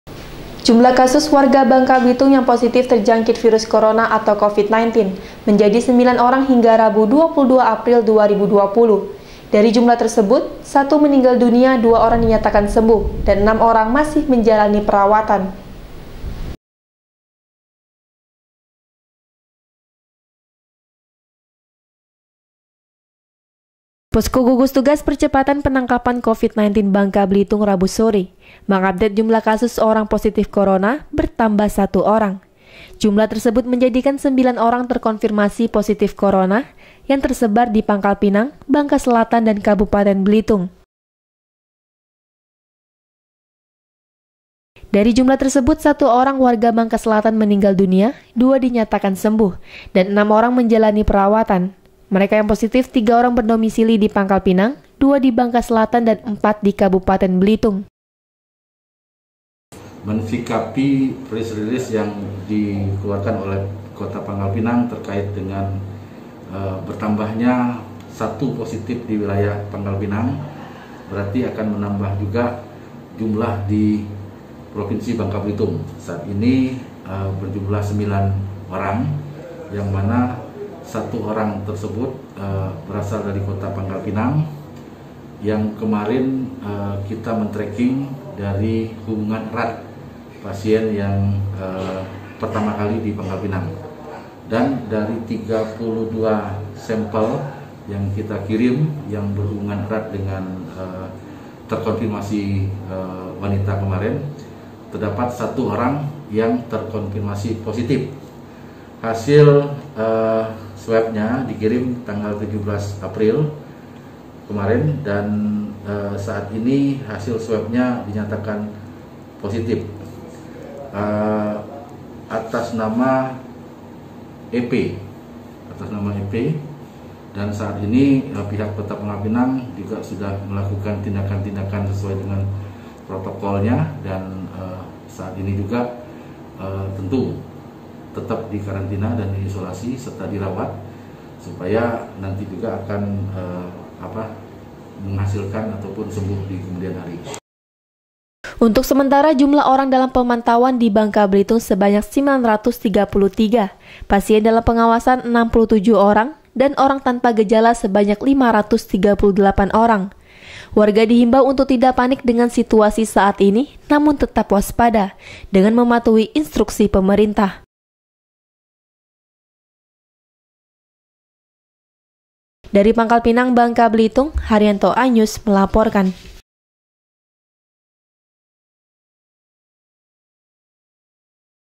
KASUS WARGA BABEL POSITIF COVID 19 JADI SEMBILAN ORANG
JUMLAH KASUS WARGA BANGKA BELITUNG YANG POSITIF TERJANGKIT VIRUS CORONA ATAU COVID-19 MENJADI SEMBILAN ORANG HINGGA RABU 22 APRIL 2020. DARI JUMLAH TERSEBUT, SATU MENINGGAL DUNIA, DUA ORANG DINYATAKAN SEMBUH DAN ENAM ORANG MASIH MENJALANI PERAWATAN.